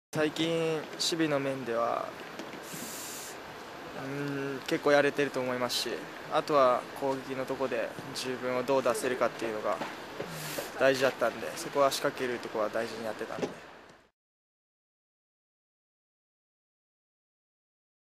２０１２宮崎キャンプFLASH5
サンフレッチェ広島２０１２宮崎キャンプ

■トレーニングキャンプ（２次）
【日程】２０１２年２月１３日（月）～２０１２年２月２６日（日）
【場所】フェニックス・シーガイア・リゾート　イベントスクエア（宮崎県）